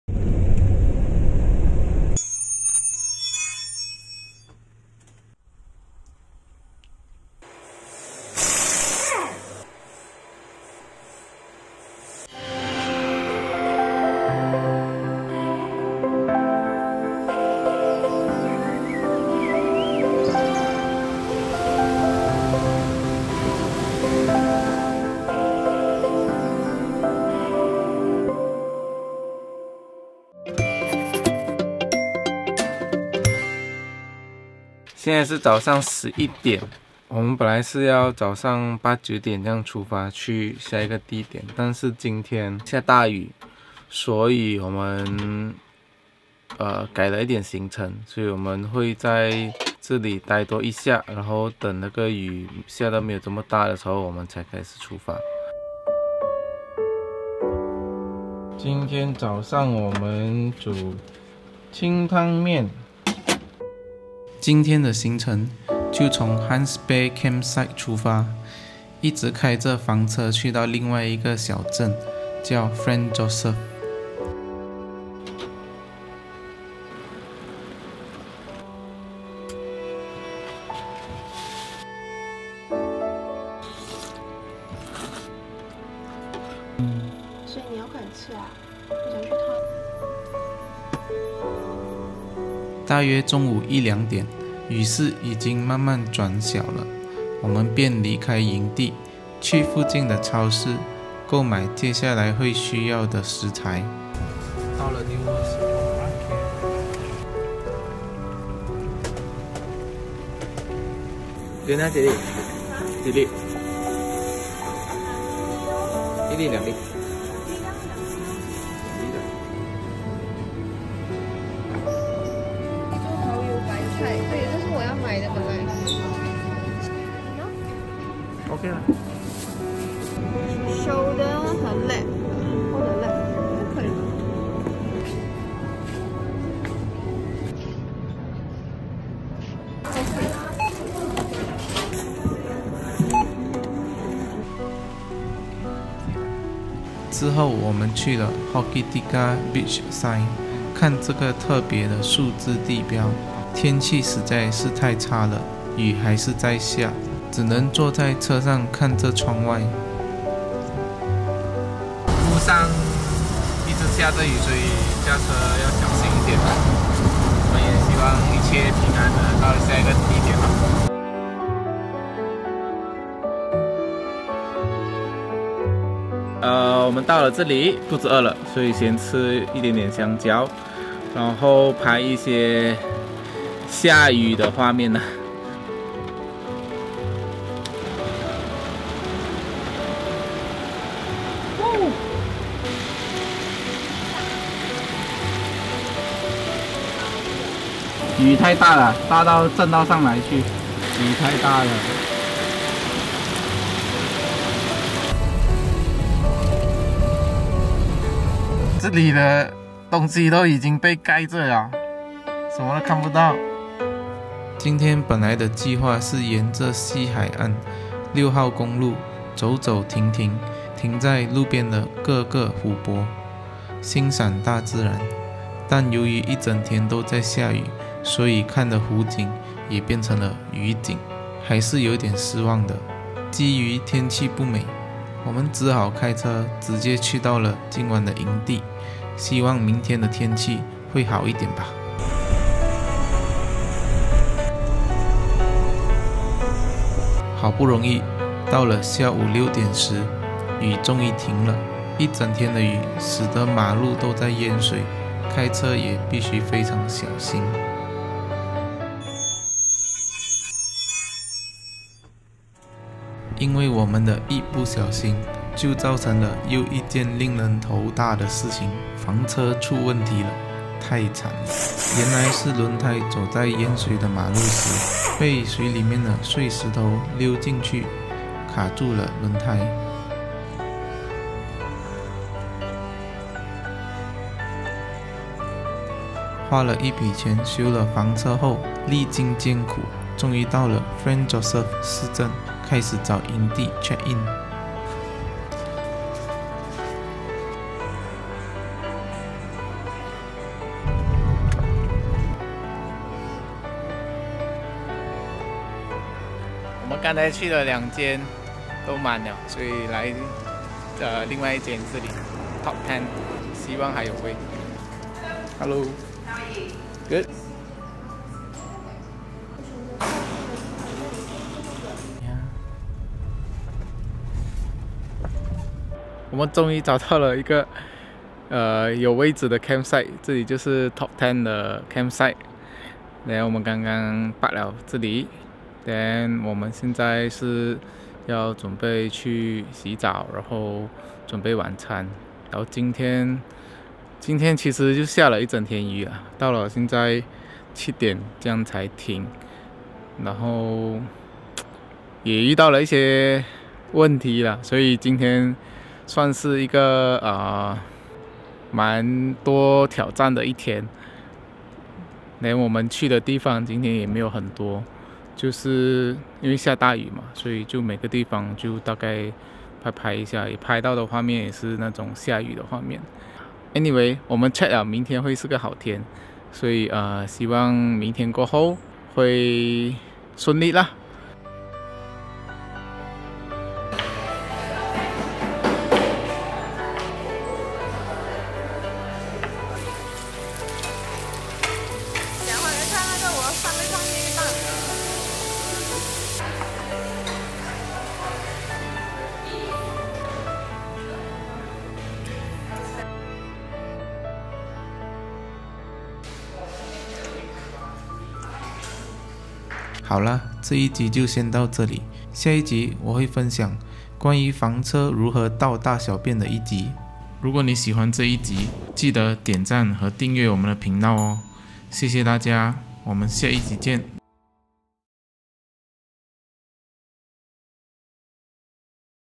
【新西兰南岛之旅 #07 - 西海岸6号公路】一整天下大雨，马路淹水把房车都弄坏了!｜ NZ South Island Vlog Ep 7
JJ 74 -【NZ South Island Vlog Ep 7 - 西海岸6号公路】一整天下大雨，马路淹水把房车都弄坏了!｜ 纽西兰/新西兰南岛之旅 #07

00:00 Intro
01:42 New World Supermarket
02:24 Hokitika Beach Sign
02:56 Lake Lanthe
03:28 Lake Mapourika
04:18 Campervan Breakdown
05:00 Top 10 Holiday Park